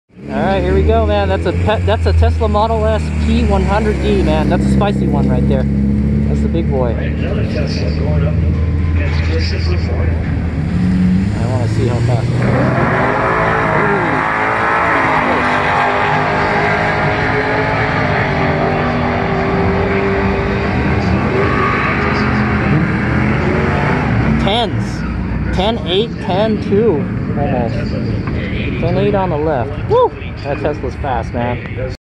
Tesla Model S P100D @ Wednesday Night Drags Sonoma Raceway 07-31-2019
10 second pass for an EV!  The Tesla Model S P100D is about as high spec as you can get.